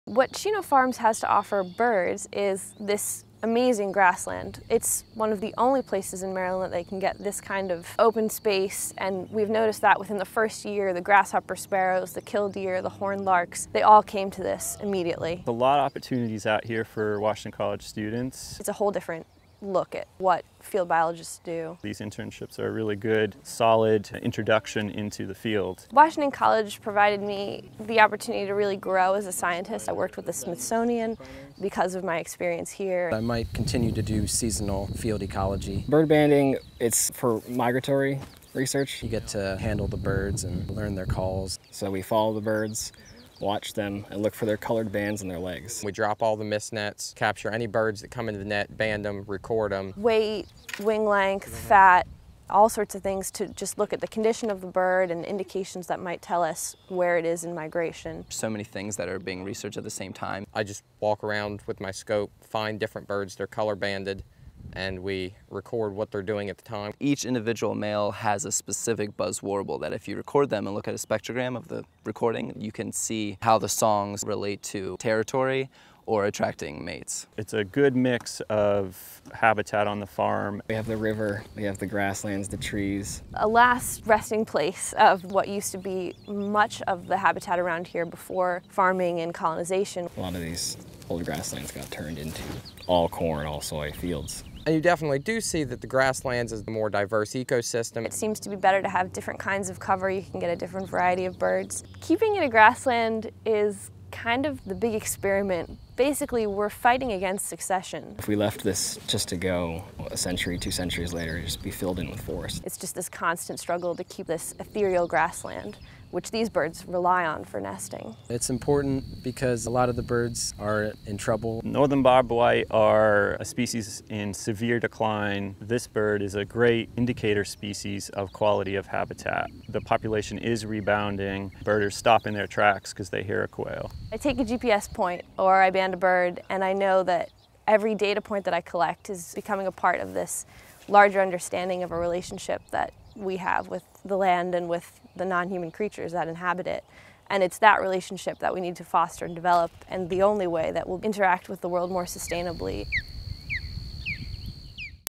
Student Research at Chino Farms
Washington College students team up with ecologists and colleagues from other institutions to study the effect of grasslands restoration on migratory bird populations at the Chester River Field Research Station.